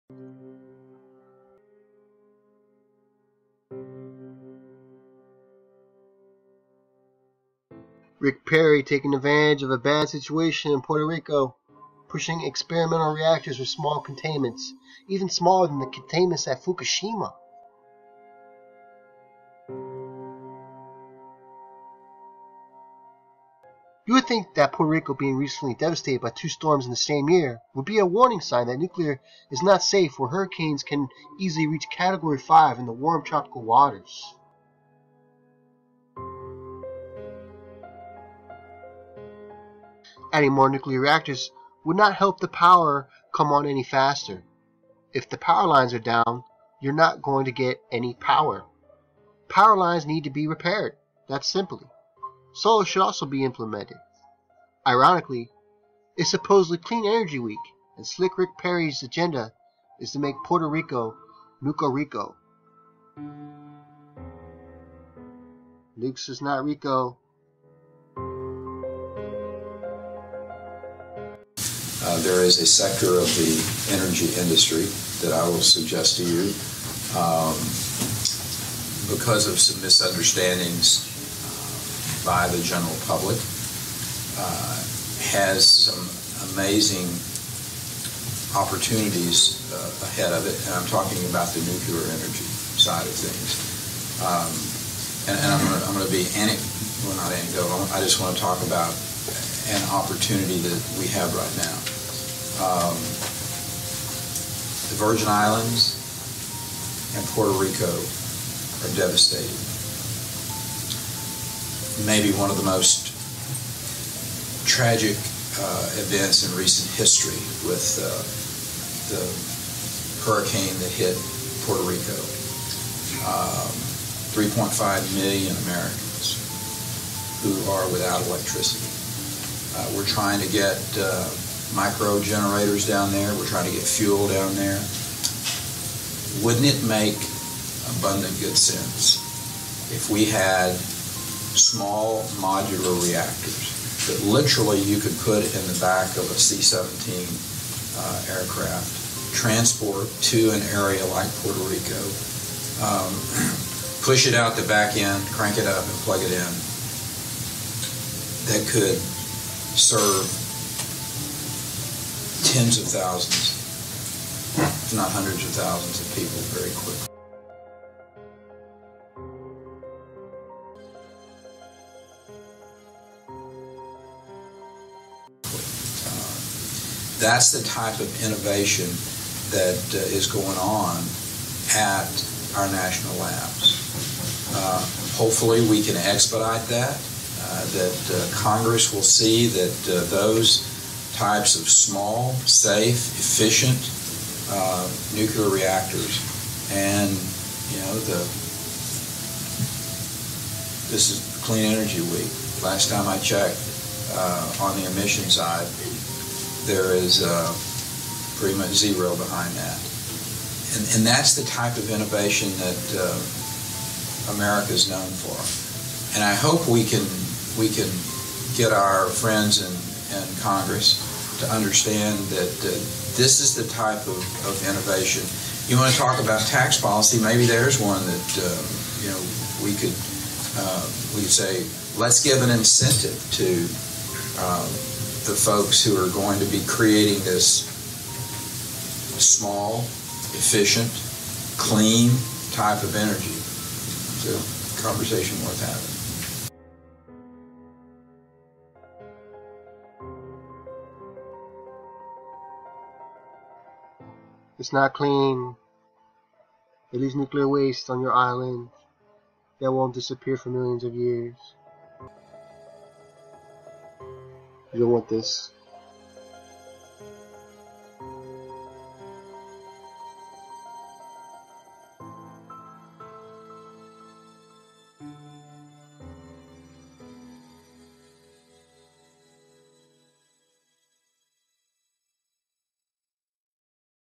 Rick Perry Pushes Puerto Rico Nuclear Says Waste Pretty Much 0 Solar Best
Rebuild Puerto Rico's grid with solar, not nuclear, In terms of speed of recovery, nothing beats solar with storage. As Inside Climate News reported, Florida homes with solar panels and batteries that lost power from Irma in the early hours of Monday, September 11 were able to restore power in a few hours when the sun came up. Long before others, they could use lights, refrigerators, and wifi.

Although it’s worth noting that solar homes without storage are out of luck in Florida. The utility, FPL, does not allow grid-connected systems without batteries to be used during an outage. And the state code requires you to be connected to the grid even if you could be grid free.

Dirty energy is not only dirty, it is very expensive for an island. Nuclear power is hazardous and expensive everywhere. Neither are resilient to extreme disaster. Renewables are fast, clean, resilient, cheap — and getting cheaper every year. They are exactly what Puerto Rico’s grid needs.

Energy Secretary Rick Perry said Tuesday that small nuclear power plants being researched at national labs are the kind of “innovation” he’d like to “expedite” for Puerto Rico’s rebuild. "If we had small modular reactors that literally you could put in the back of C-17 [military cargo] aircraft, transport it to an area like Puerto Rico, and push it out the back end, crank it up, and plug it in?” he said at an event in Washington, D.C. for National Clean Energy Week. “Hopefully, we can expedite that.”